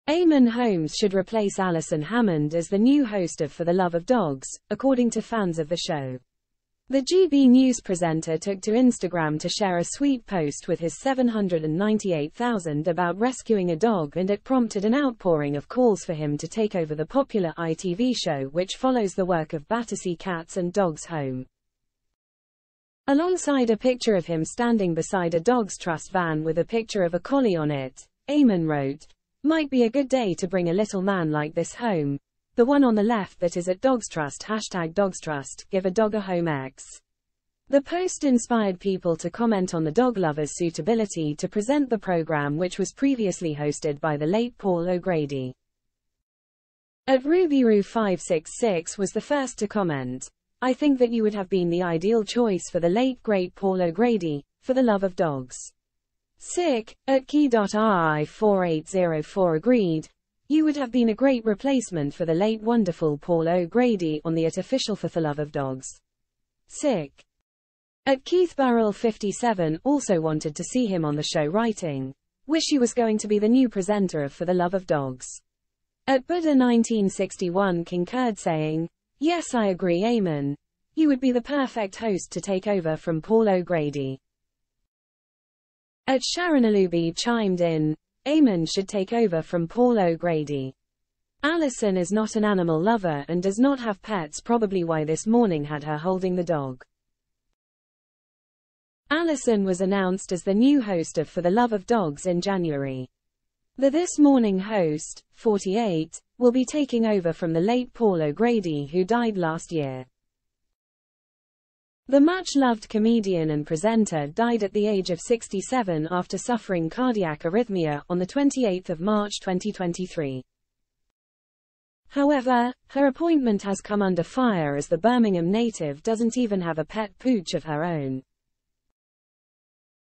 ITV fans call for Eammon Holmes to 'take over' from Alison Hammond on popular show
Eamonn Holmes should replace Alison Hammond as the new host of For The Love Of Dogs, according to fans of the show. The GB News presenter took to Instagram to share a sweet post with his 798,000 about rescuing a dog and it prompted an outpouring of calls for him to take over the popular ITV show which follows the work of Battersea Cats and Dogs Home.